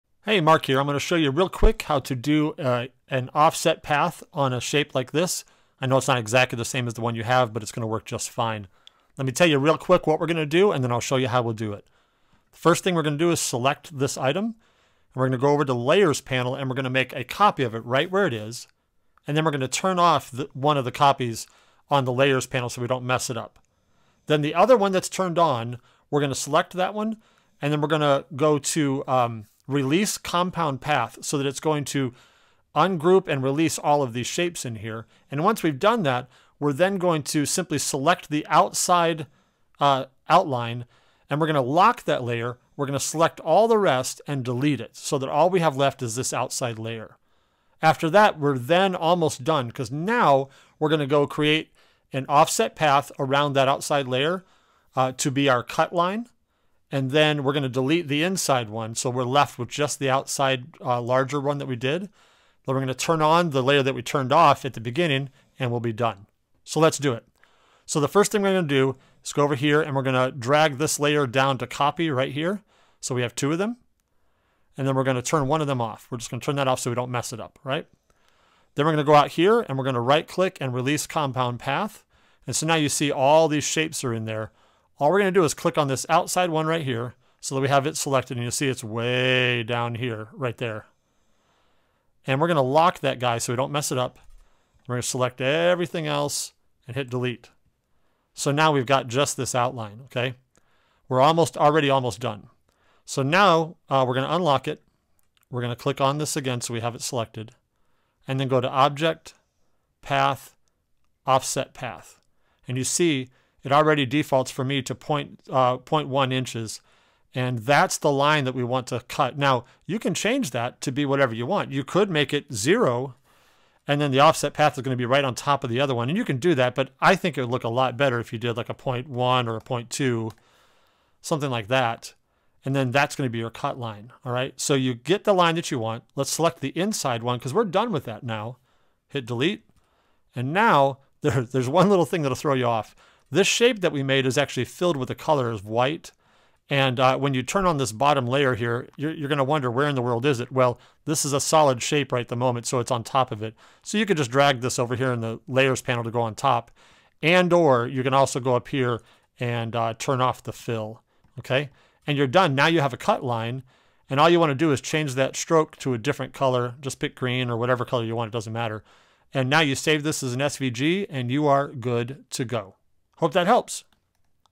Tutorial: Adding an Offset Path to a Decorative Letter (Illustrator)
I had a few people want me to show them how to add a cut line to a design that is all one shape, so here I show how to do that quickly in Illustrator. Adding an offset path allows you to create an outline that you then make a different color that the Glowforge will recognize.